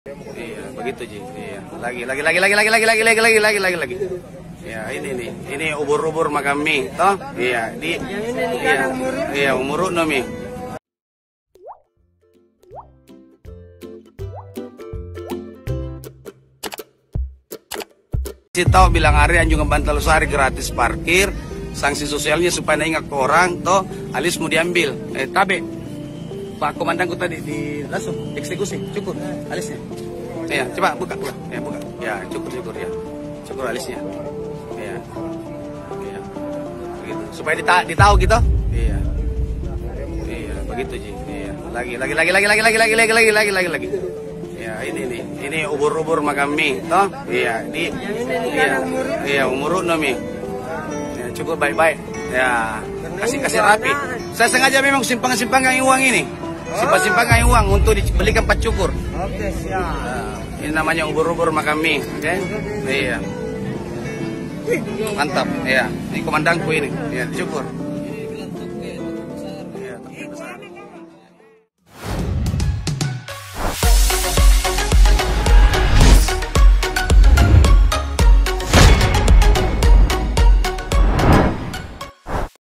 Dua Pemuda Lakukan Parkir Liar di Pantai Losari Makassar, Dapat Sangksi Alis Dicukur Habis Oleh Petu
Dua pemuda di anjungan Pantai Losari, Kota Makassar dicukur habis alisnya oleh aparat gabungan dari Satuan Polisi Pamong Praja (Satpol PP) dan Polsek Ujung Pandang pada Sabtu (31/10/2020) malam. Keduanya kedapatan memalak dan melakukan pungutan liar terhadap pengunjung yang memarkir kendaraannya di lokasi wisata ikonik Kota Daeng itu.

Kepala Satpol PP Kota Makassar, Iman Hud mengatakan, kedua pemuda itu diamankan oleh petugas gabungan saat pengamanan di Anjungan Pantai Losari, pada Sabtu 31 Oktober 2020, malam. Mereka tertangkap tangan, tengah melakukan pengutan liar terhadap pengunjung dengan modus biaya parkir.

"Mereka diamankan karena minta biaya parkir kepada pengunjung. Padahal biaya parkir di Pantai Losari itu gratis. Sehingga, kedua pemuda ini diberikan sanksi sosial (cukur alis), biar jera," kata Iman Hud, Senin, 2 November 2020.

Iman menjelaskan, pengunjung kawasan wisata Anjungan Pantai Losari sebenarnya sudah tidak lagi dibebankan biaya parkir sejak tahun 2015. Sehingga, barang siapa yang meminta biaya parkir kepada para pengunjung merupakan perbuatan pungutan liar.

(TRIBUN-TIMUR.COM)